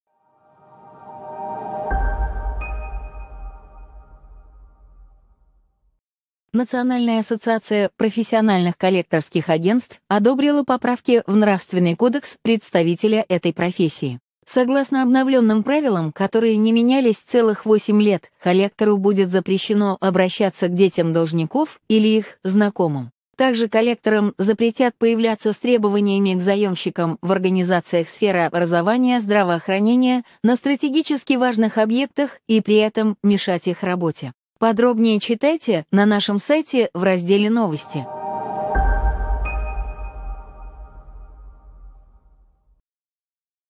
Коллекторы обновили свой нравственный кодекс
НАПКА одобрила поправки в нравственный кодекс представителя этой профессии. Важно, что правила не менялись целых 8 лет. Что изменилось? Читайте далее